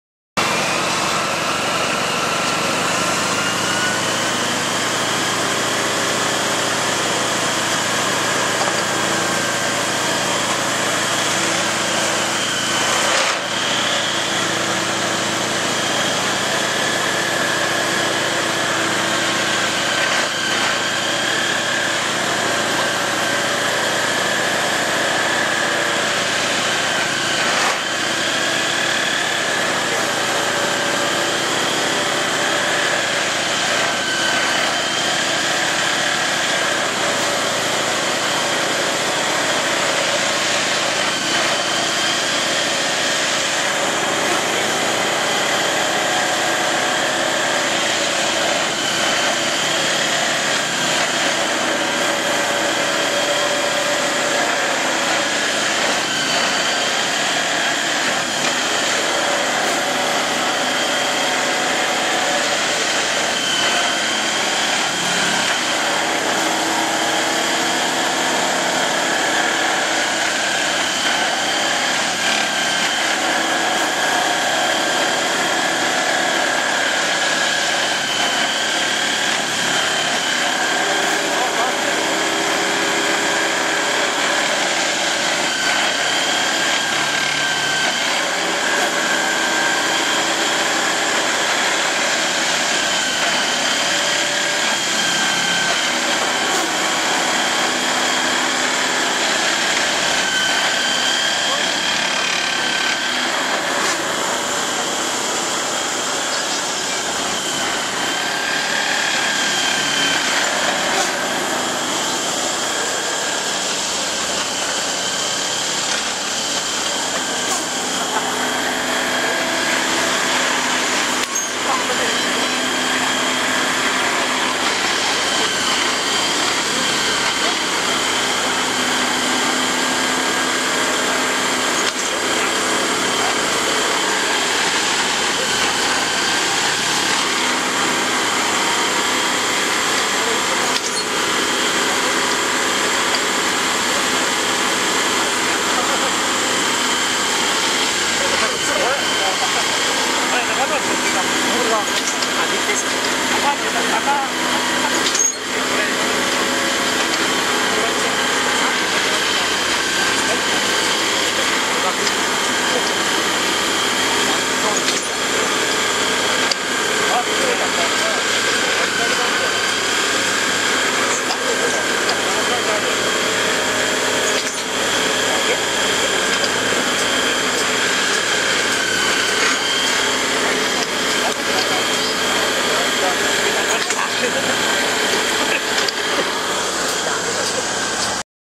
ＪＲ高崎マルタイ実演動作２０１５
平成２７年１０月３１日、「高崎鉄道ふれあいデー」にて実演された、マルタイの動作実演の様子。